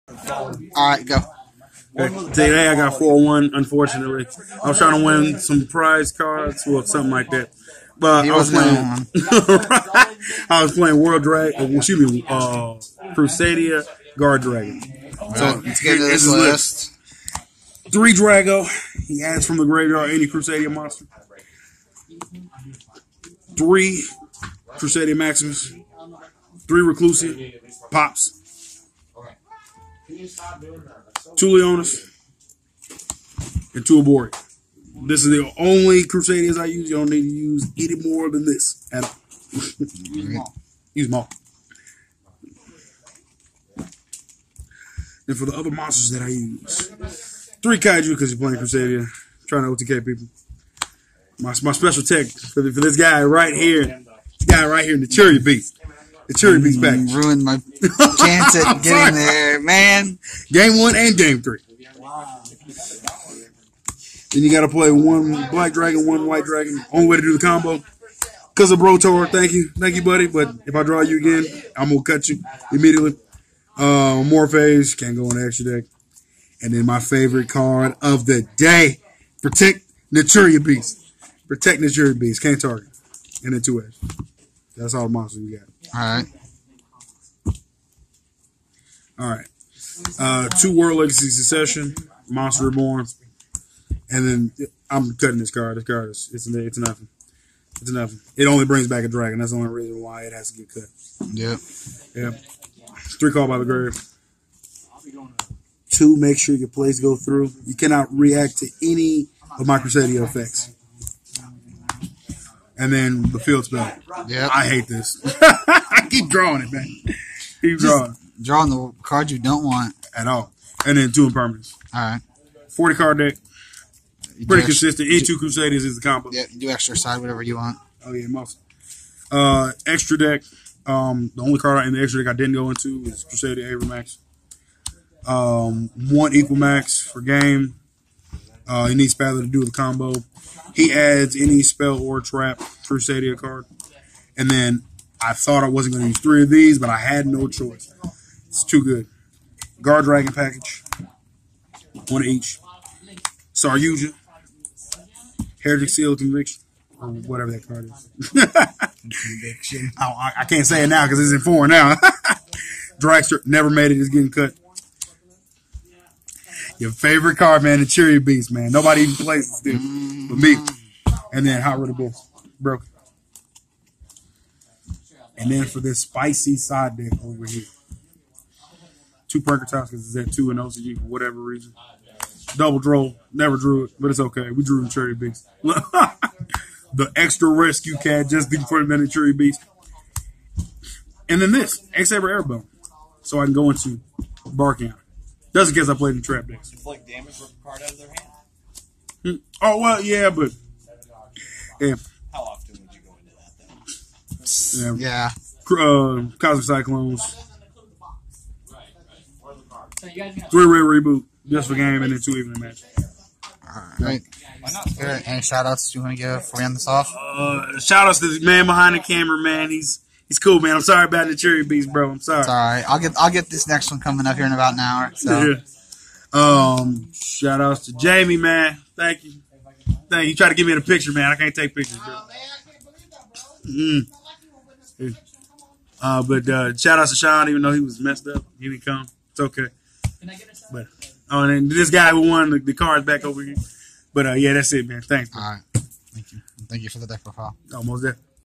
Yu-Gi-Oh Worlds Celebration 2019 3rd Place Deck Profile: Crusadia Guardragons!
Alright guys got a deck profile of the guy who got 3rd place at this weekends worlds celebration unfortunately losing the last round but still did well!